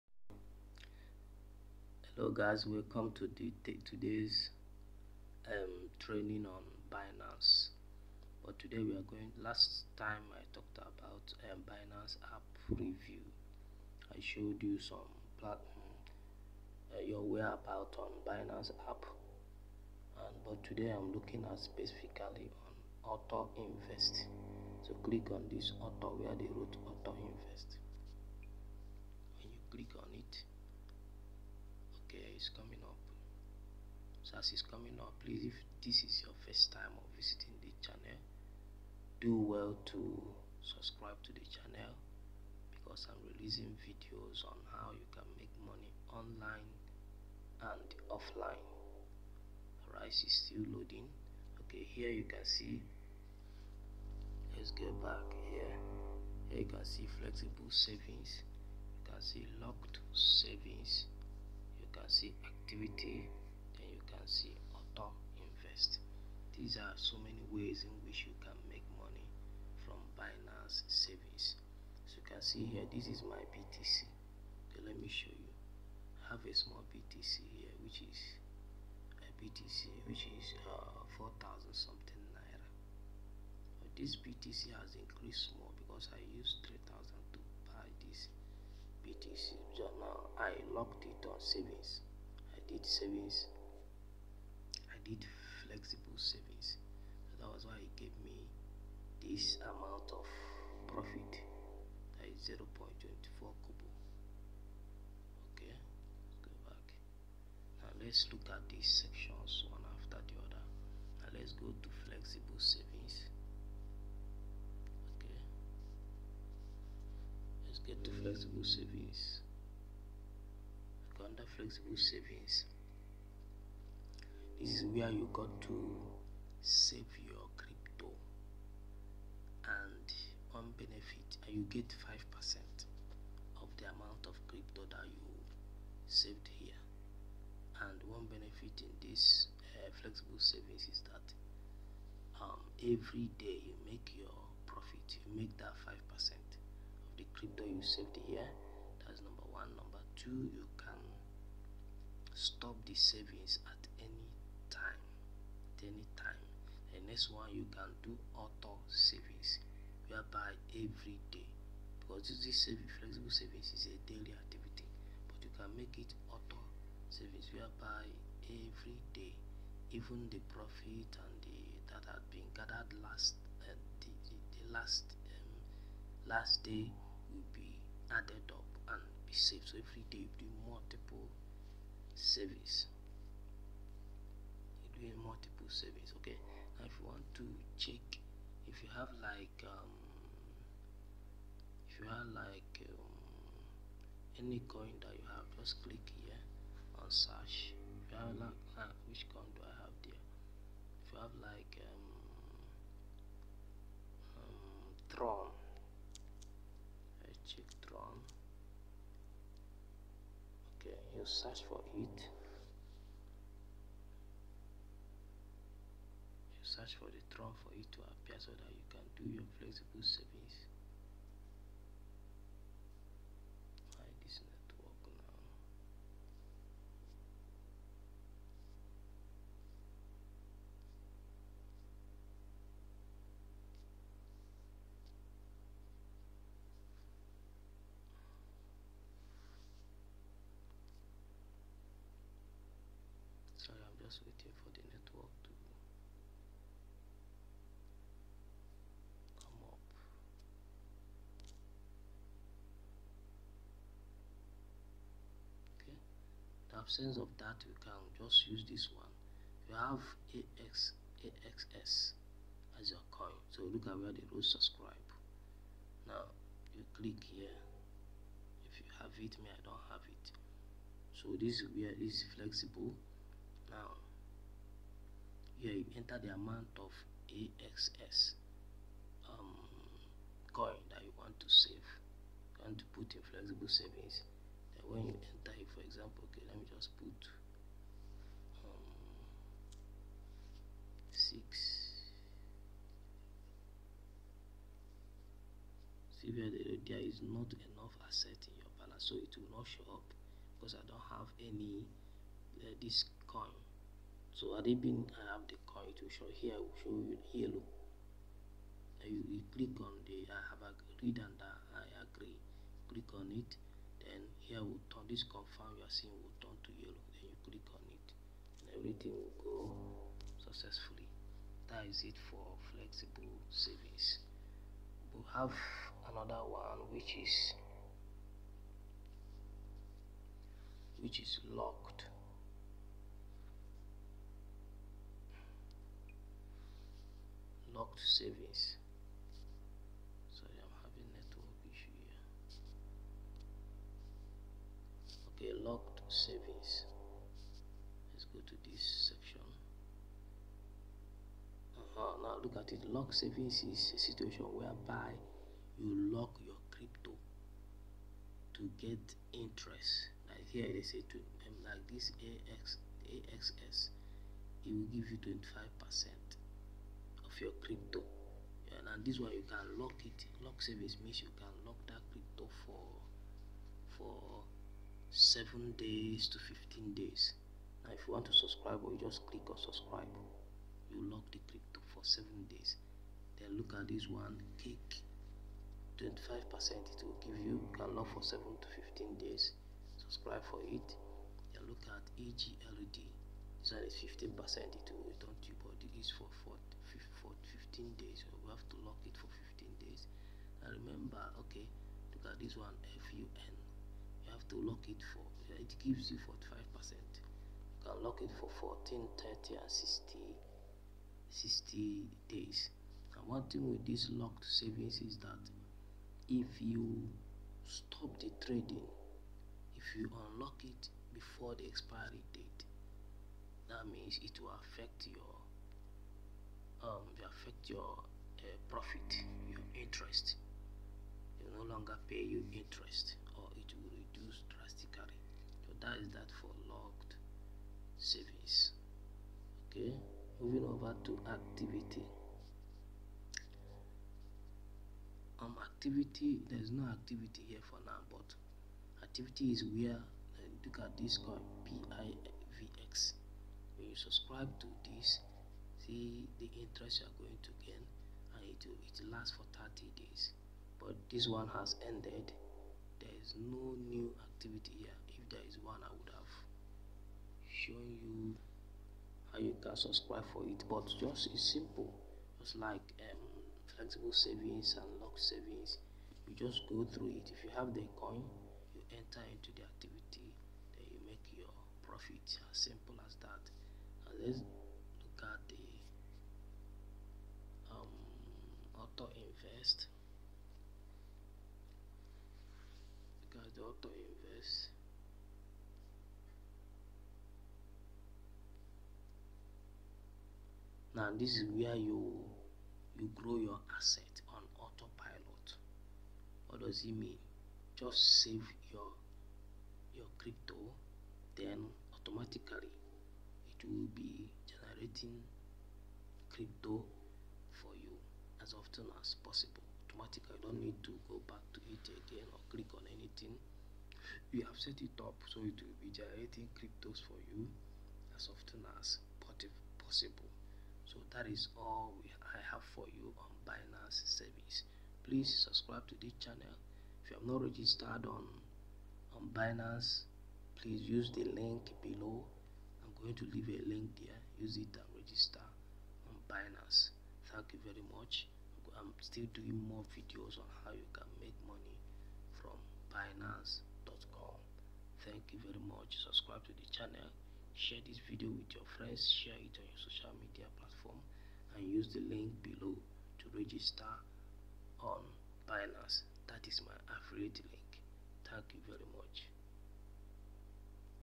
Binance Auto Invest I Binance IBitcoin I Binance Earn
In this video you are going to learn how to do binance auto invest